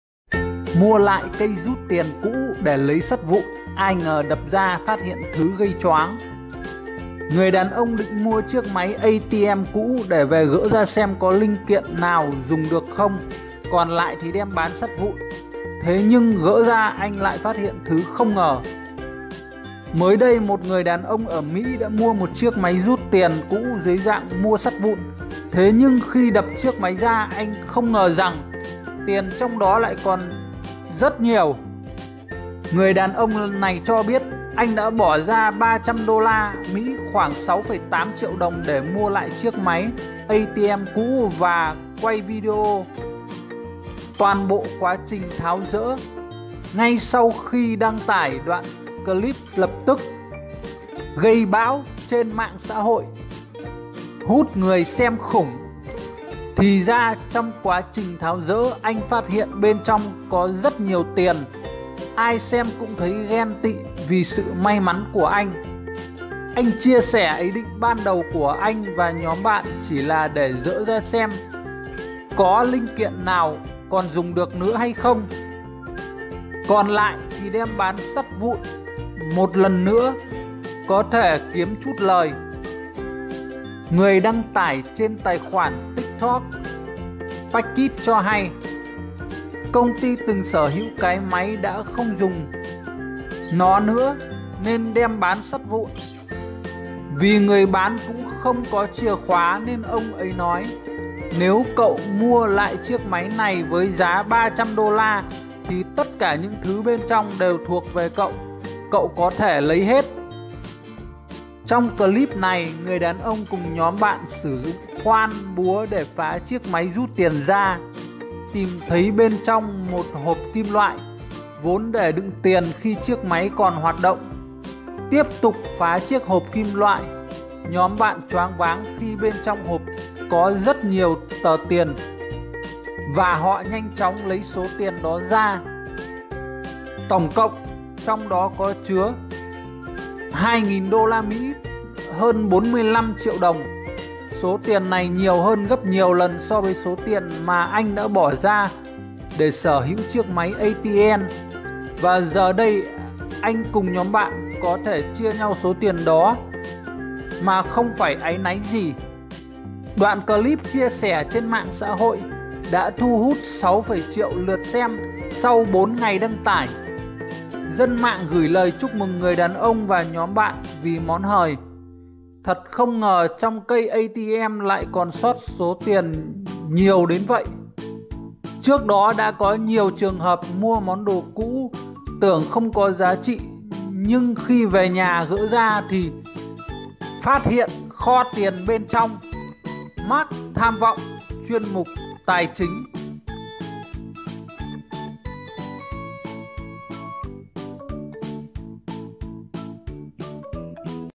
Mua lại cây rút tiền cũ để lấy sắt vụn, ai ngờ đập ra phát hiện điều bất ngờ
By: Marc Tham Vọng.